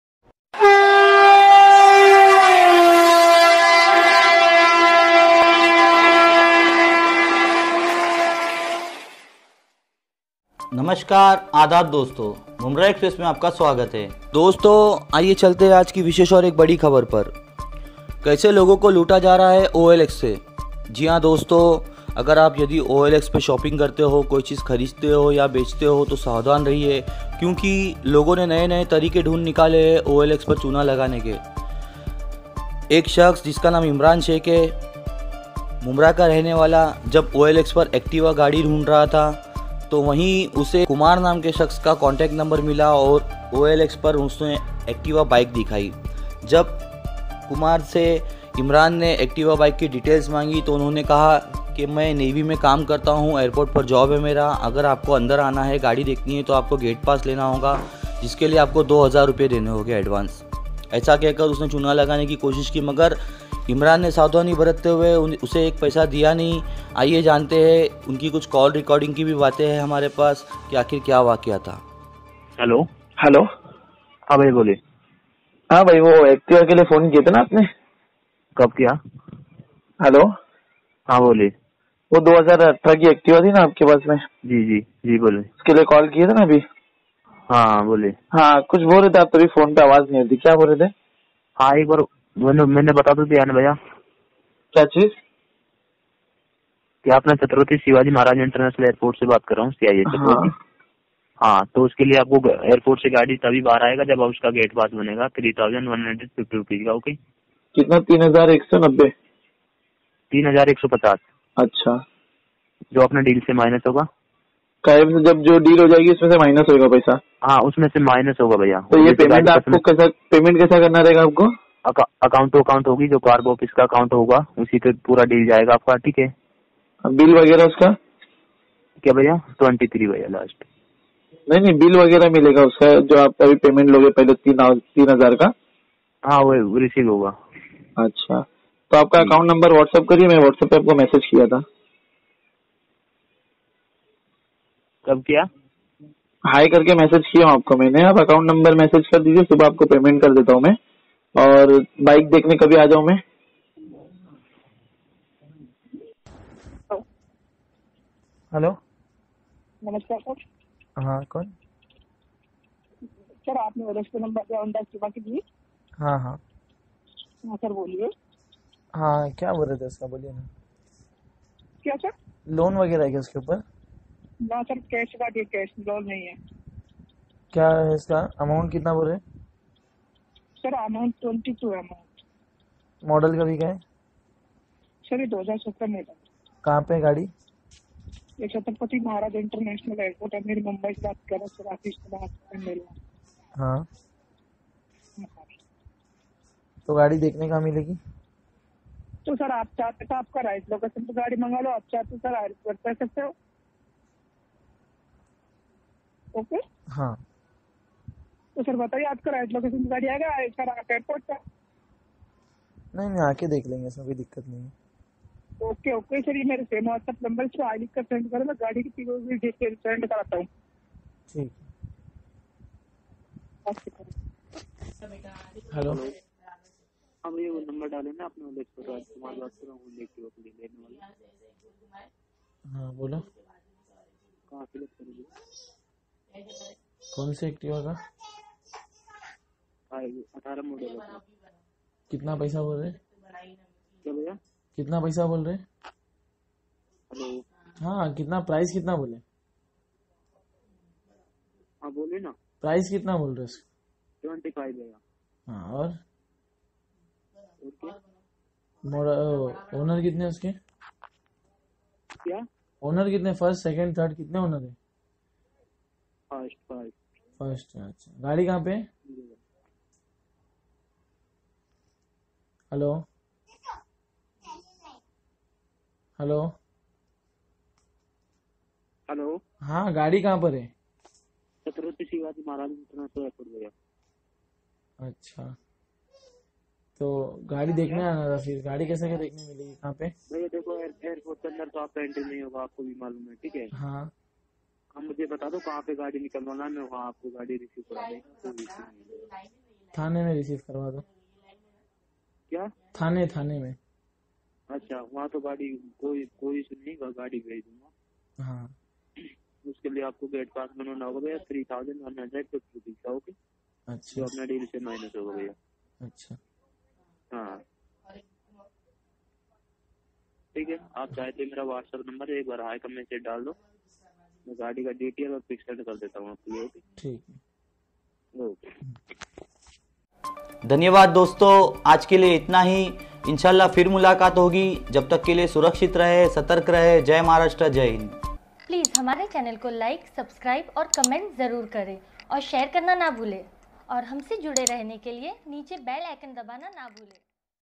O.L.X pe Se Shopping karnewalo ko kaise loota ja raha hai ? | Me.Tv News
Dosto dekhiye kis tarah se loota ja raha hai logo ko OLX ke zariye ..

Janiye aur dekhiye kya hua poori jankari ..
Produced by : Mumbra eXpress News
Edited by       : Salman Ansari
On screen      : Rafshan Pirani
Alert viewer :